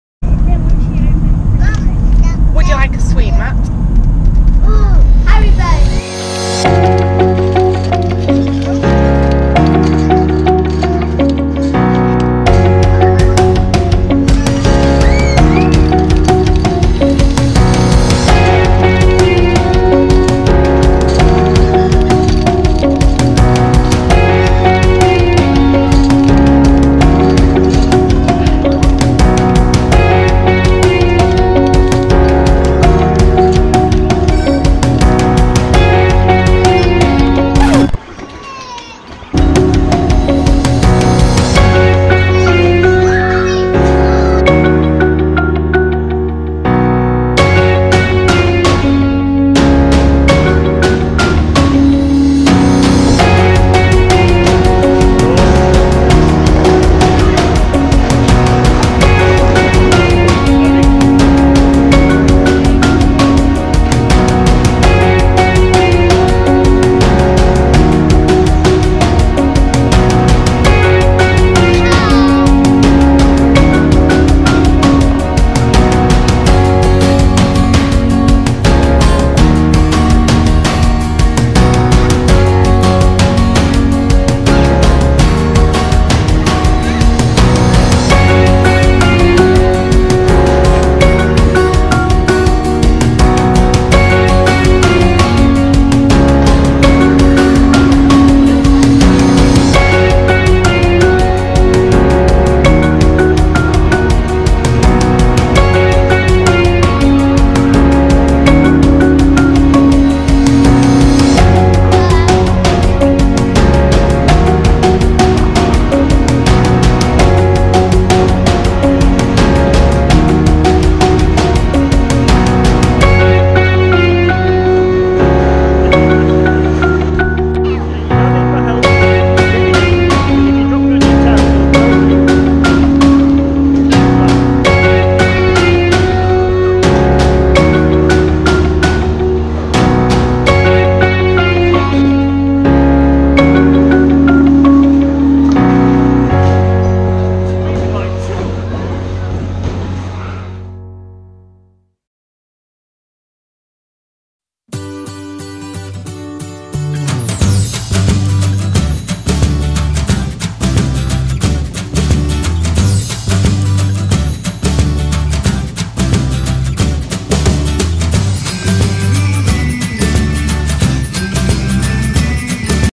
Bowling - The Gilberteenies E6
Today we visited york ten pin for a bowling session!